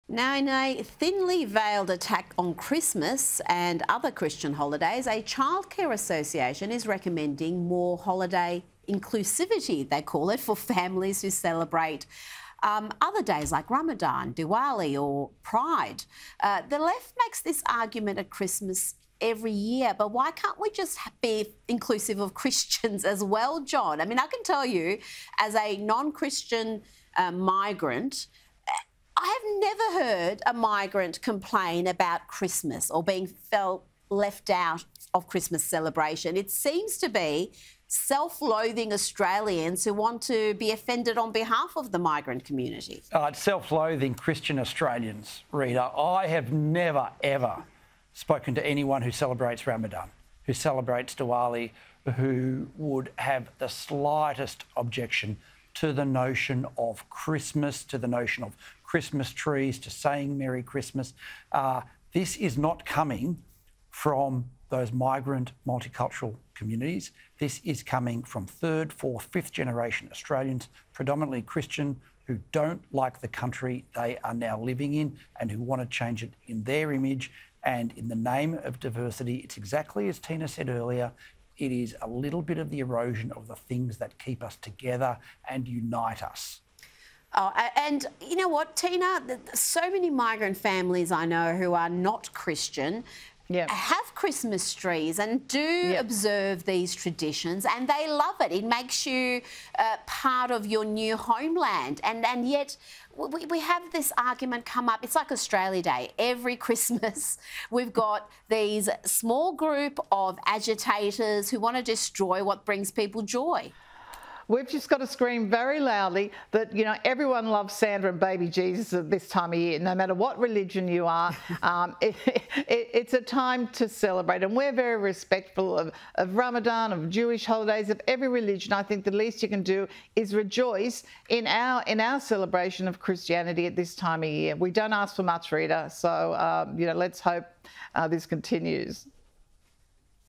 ‘In the name of diversity’: Push for the inclusion of holidays other than Christmas
The Institute of Public Affairs' John Roskam says the push for holidays other than Christmas to be celebrated is coming from predominately Christian Australians as opposed to migrant multicultural communities.

“In the name of diversity … it is a little bit of the erosion of the things that keep us together and unite us,” Mr Roskam told Sky News host Rita Panahi.